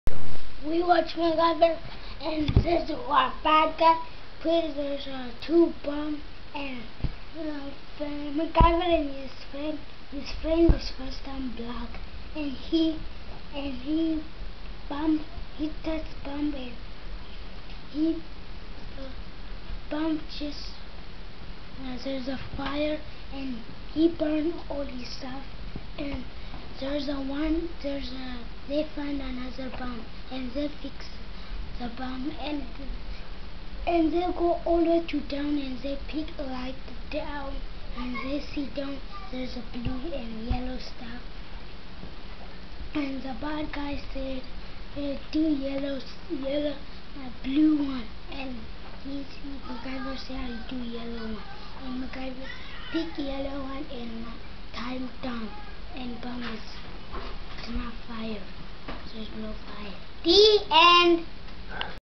AJ Explains a MacGyver Episode
Our nine year old really liked the last MacGyver he watched.